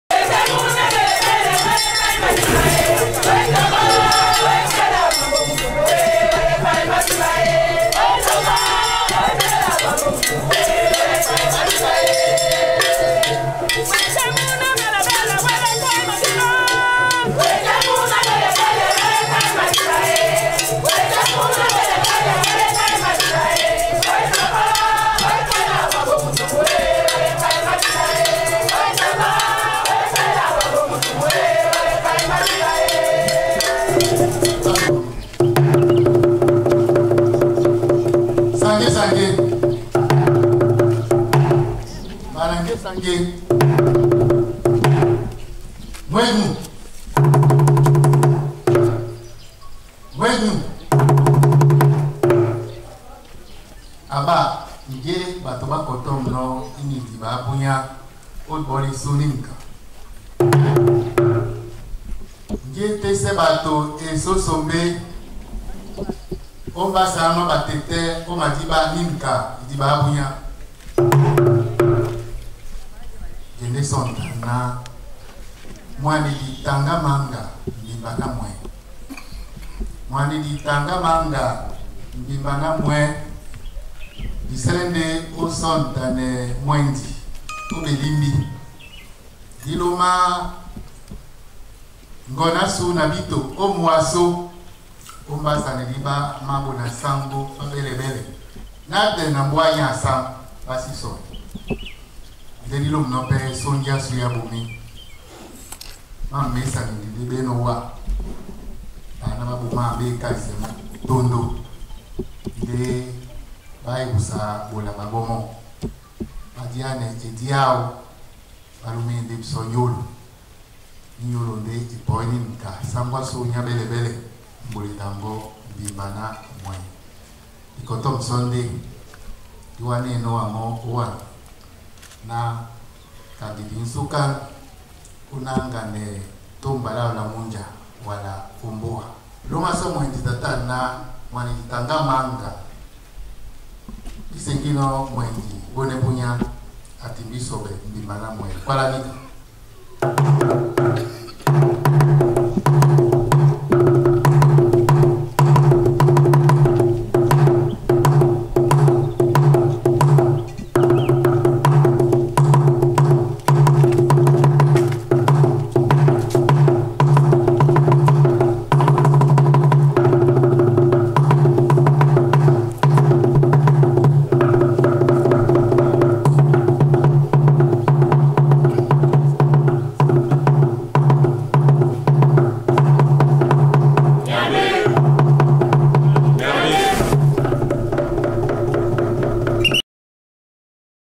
Sa Majesté TANGA MANGA Alfred Chef de JEBALE 1
Les derniers hommages à SM TANGA MANGA Alfred organisés et célébrés par tout le peuple Sawa et la communauté JEBALE sur les berges du fleuves Wouri avant la traversée de sa dépouille mortuaire pour son village natal.